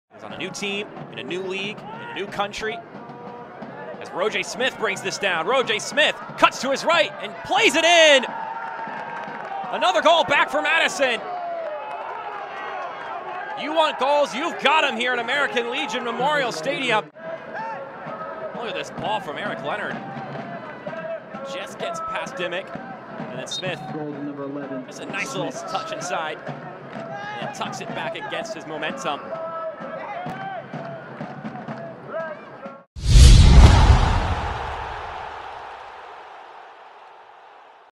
Rojay Smith with a Goal vs. Charlotte Independence
Rojay Smith (Forward Madison FC) with a Goal vs. Charlotte Independence, 06/26/2022